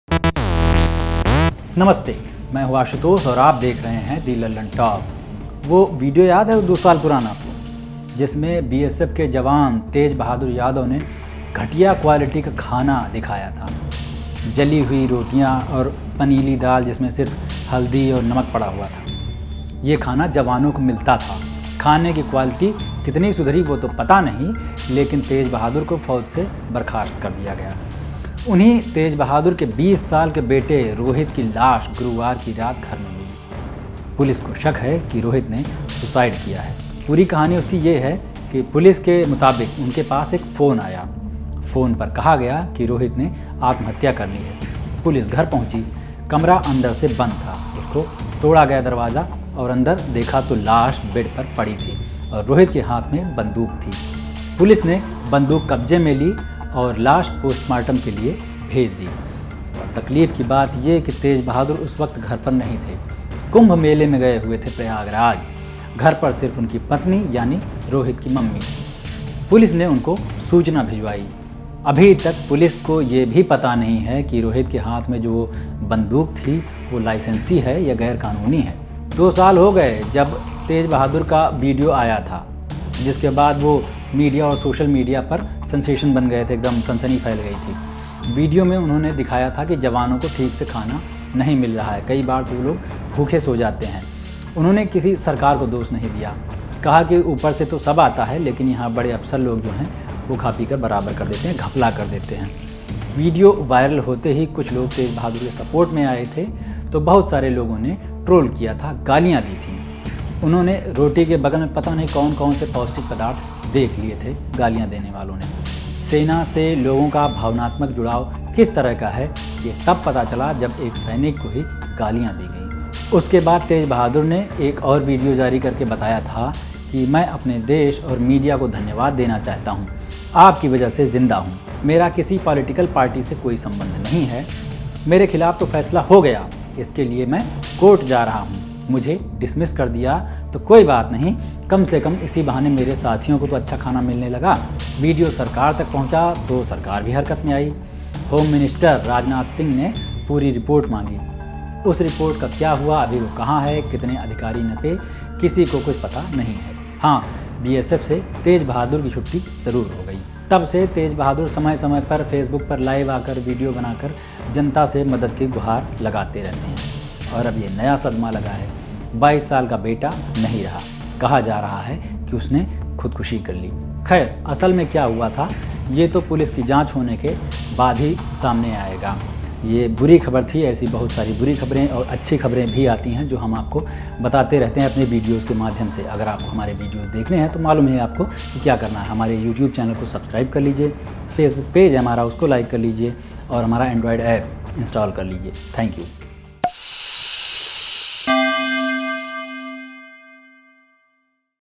BSF से बर्खास्त जवान Tej Bahadur Yadav के बेटे की लाश हाथ में पिस्टल लिए बेड पर मिली
BSF constable Tej Bahadur Yadav who was dismissed from service after he had released a video last year on quality of food served to soldiers. His 22 years old son Rohit found dead at his residence in Shanti Vihar, Rewari.